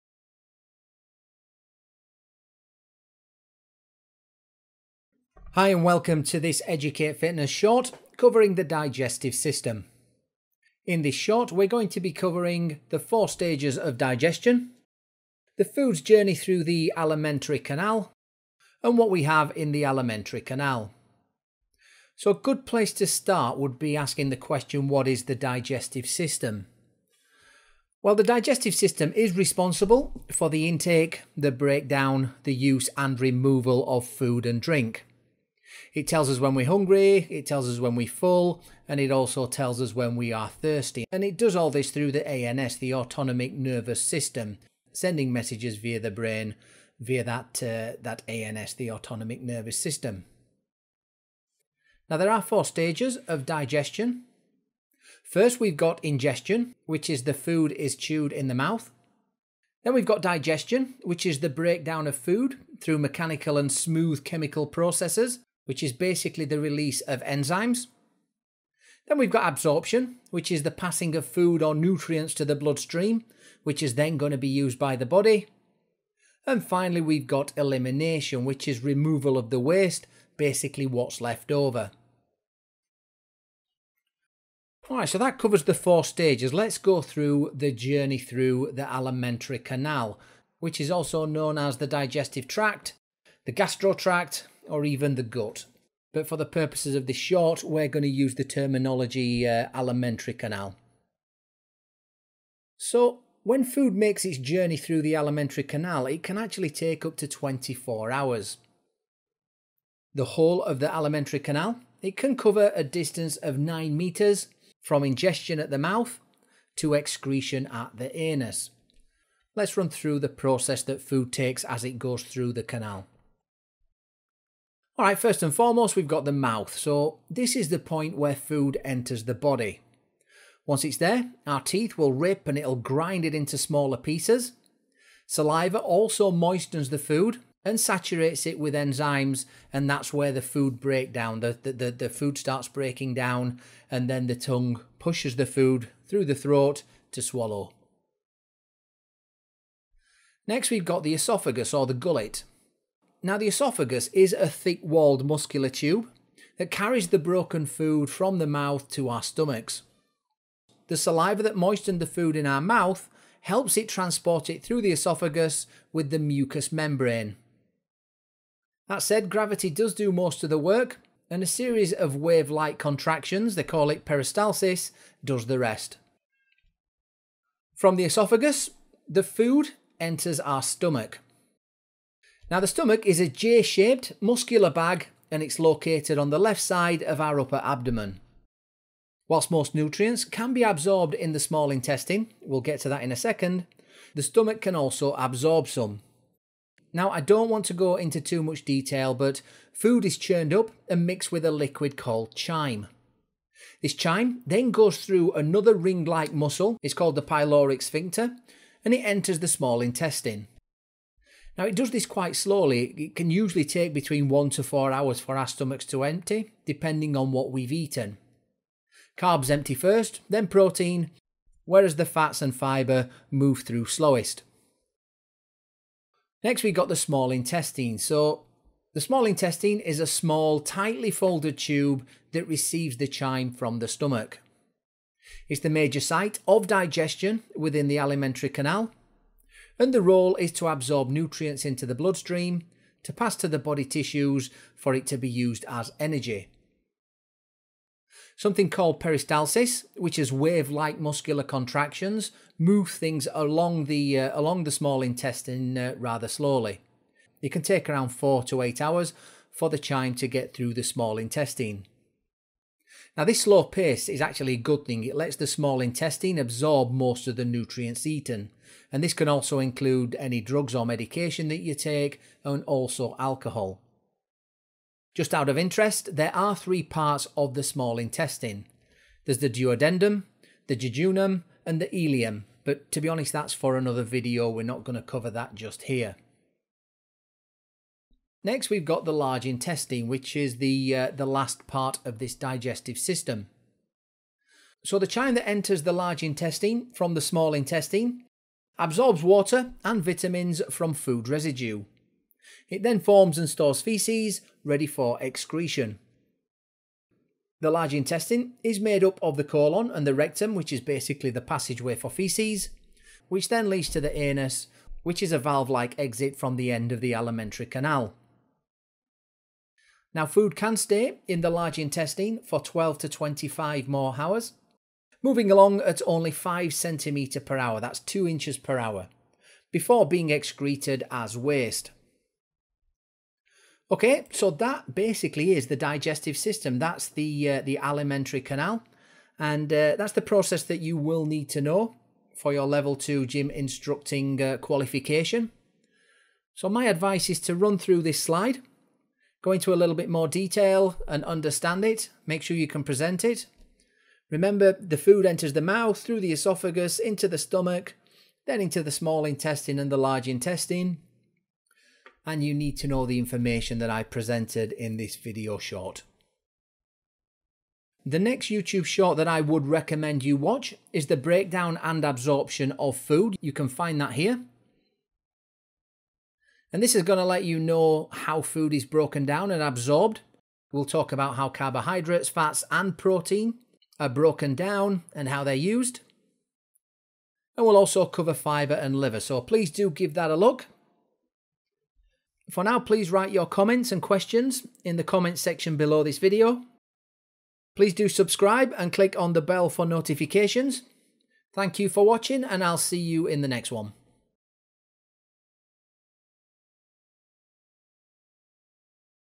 The Digestive System - L2 Gym Instructing
Health and Fitness consultant Damian Foster takes you through a brief explanation of 'The Digetsive System', which is required as part of the Level 2 Certificate in Gym Instructing.

What is the Digestive System?
- Responsible for intake, breakdown, use and removal of food and drink
- Tells us we're hungry, full & thirsty
- Sends messages to the brain via the Autonomic Nervous System (ANS)

Four stages of Digestion
1. Ingestion
2. Digestion
3. Absorption
4. Elimination

Journey through Alimentary Canal

Mouth
- Point food enters the body
- Teeth rip and grind it into smaller pieces
- Saliva moistens food/saturates with enzymes
- Tongue pushes food throat to swallow

Esophagus
- Thick-walled muscular tube
- Saliva moisten food in the mouth, transports
- Gravity does most of the work
- Peristalsis does the rest

Stomach
- Absorb some nutrients
- Food churned & mixed with Chyme
- Enters the small intestine (pyloric sphincter)
- 1-4 hours for the stomach to empty
- Carbs empty first, then protein
- Fats and fibre  move slowest

Small intestine
- Receives chyme from the stomach
- Major site of digestion
- Role to absorb nutrients
- 4-8hrs to travel through

Large intestine
- Absorbs water and vitamins from food residue
- Forms and stores faeces
- Made up of colon and rectum
- Leads to the anus
- Food can stay in LI for 12-25 more hours

Talk to an Educate Fitness consultant today and get the help you need on your health and fitness journey. Consultants are available Monday through Friday from 9am to 8pm GMT.